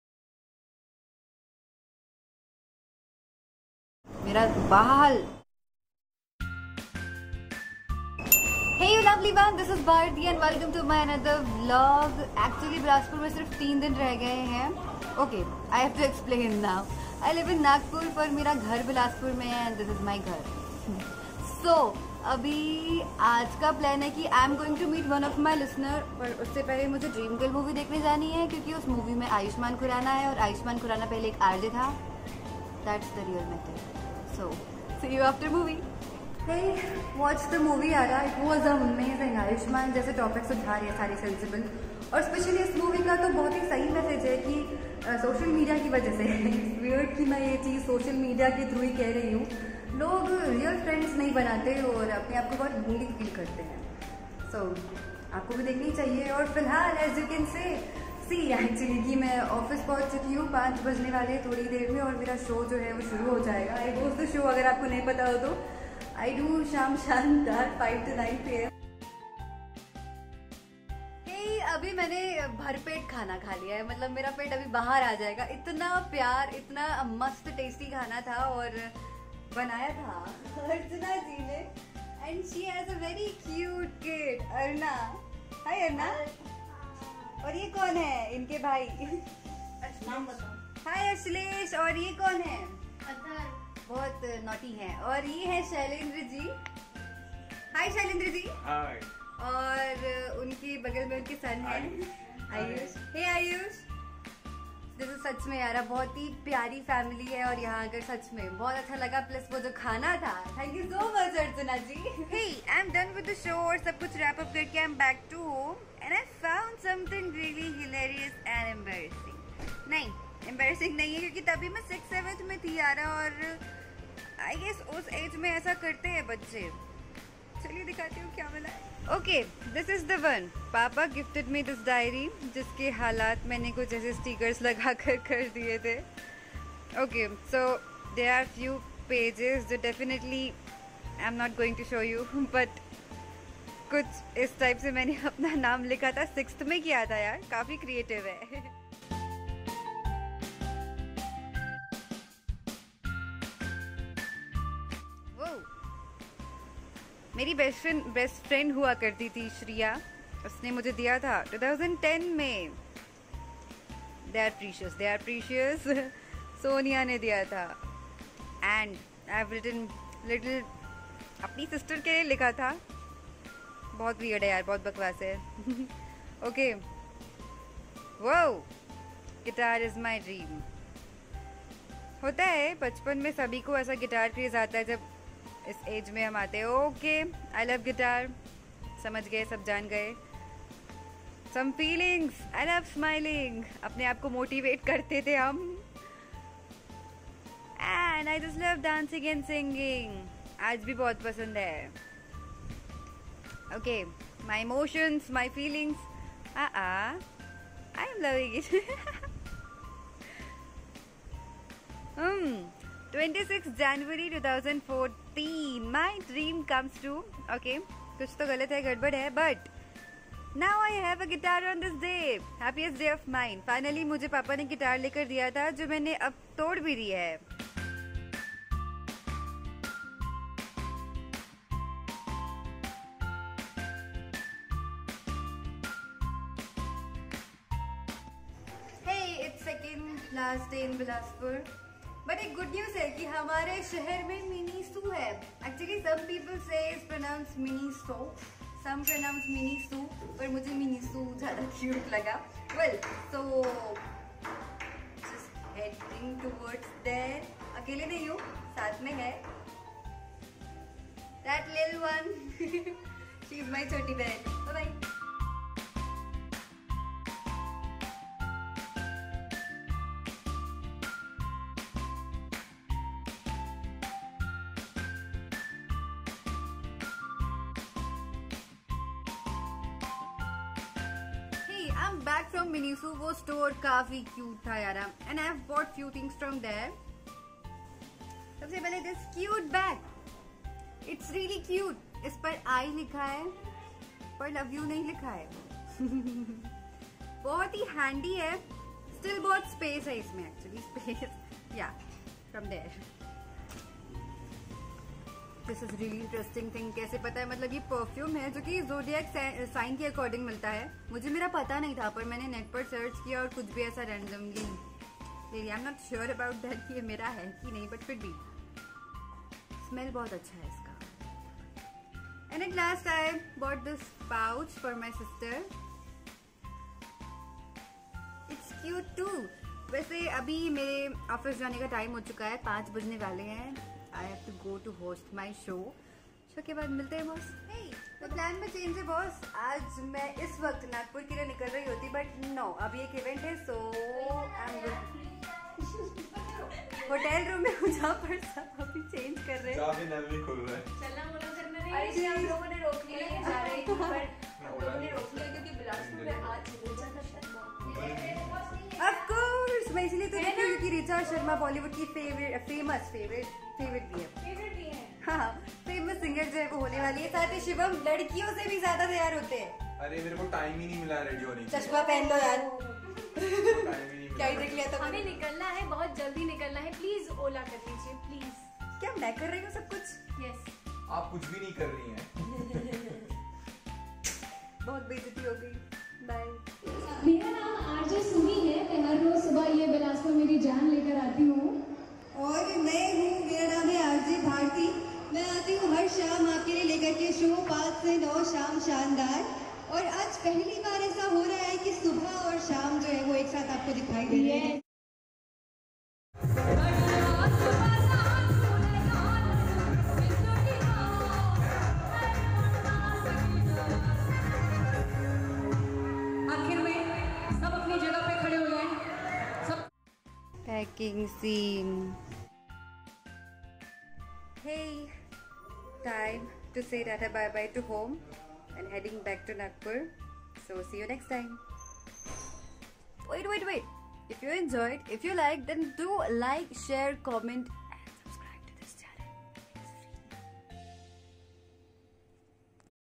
Day in my life vlog! | Meeting my listeners | Miniso in my town | Richa Sharma show | Rj Bharti
Hey you lovely1 ! this is some fun time i spent in my home in my last 3 days. meeting Richa Sharma Ji was awesome, miniso in my town was sooooo awaited thing..(loved it) and yes the most precious thing was meeting my listeners ...it was price less. so here i am sharing my lovely experience...i hope you love watching it...and if you did then do like, share, comment & subscribe to this channel...